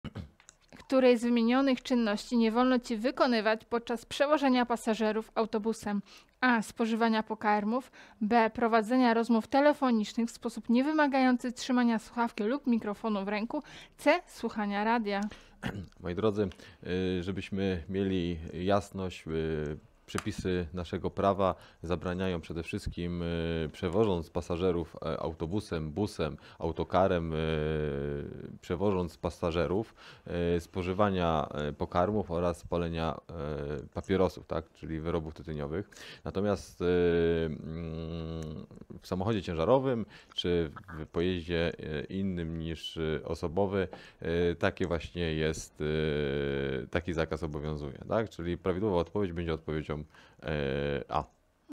Której z wymienionych czynności nie wolno Ci wykonywać podczas [pełna treść w opisie] (6389)
W tym filmie omówimy, co należy wiedzieć o przewozie pasażerów autobusem. Obejmuje to, czego nie wolno robić, a także kilka najlepszych praktyk, aby zapewnić pasażerom bezpieczeństwo i komfort.

Której z wymienionych czynności nie wolno Ci wykonywać podczas przewożenia pasażerów autobusem?

00:00 treść pytania
00:16 odpowiedź

🔗Testy na prawo jazdy dostępne na naszej stronie!